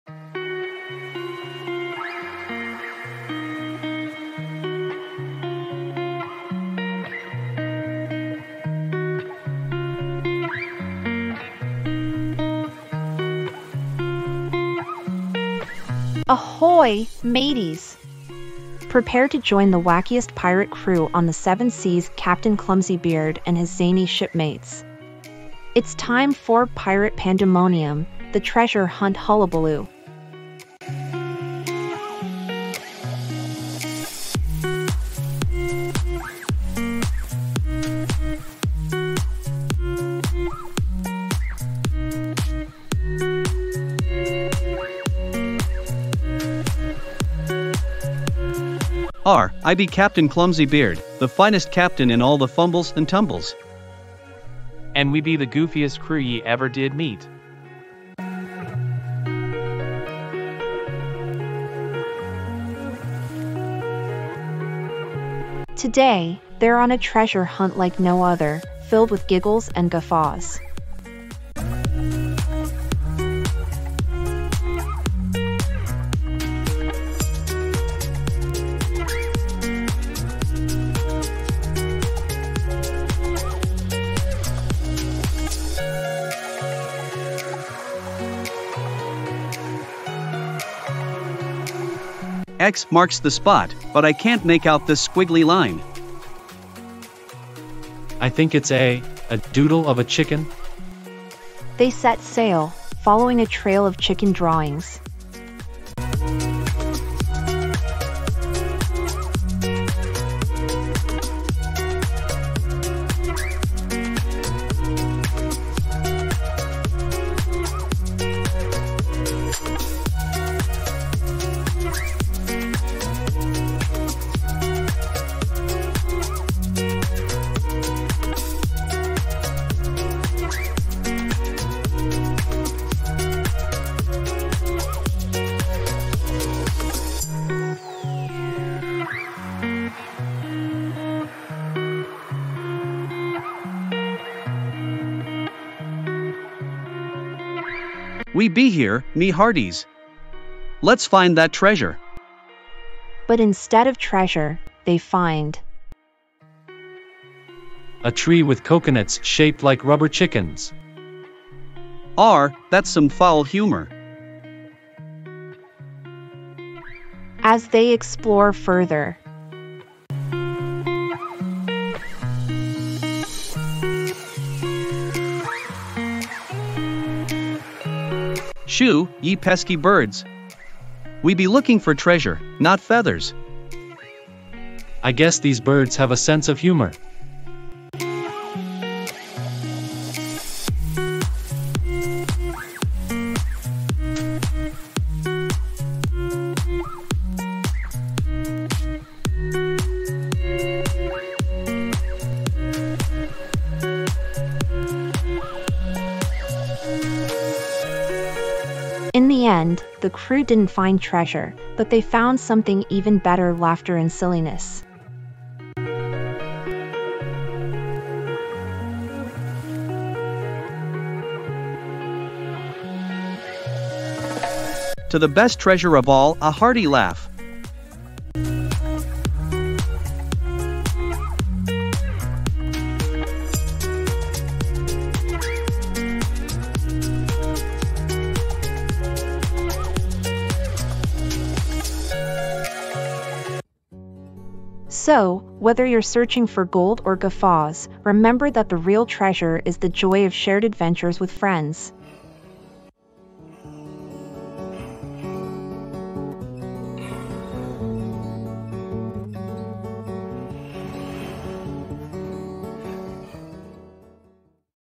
Pirate Pandemonium: The Treasure Hunt Hullabaloo
Join Captain Clumsybeard and his merry crew for a swashbuckling adventure in 'Pirate Pandemonium: The Treasure Hunt Hullabaloo'! Set sail on the high seas as they embark on a quest for treasure, encountering hilarious mishaps and silly surprises along the way. Get ready for a treasure hunt like no other, filled with laughter, camaraderie, and plenty of pirate pandemonium!